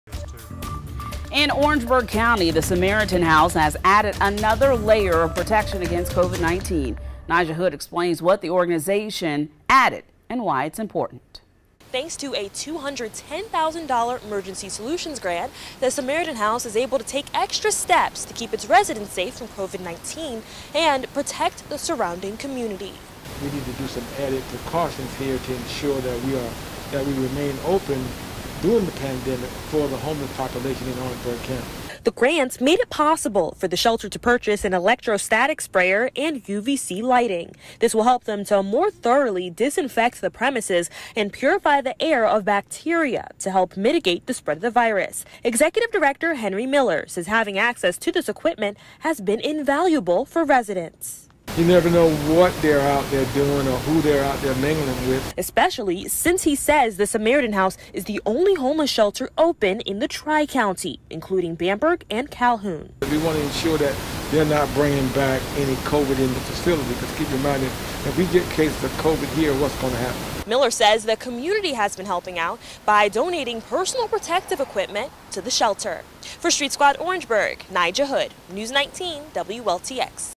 New coronavirus protections at Orangeburg's Samaritan House
The Samaritan house is the only homeless shelter in the area.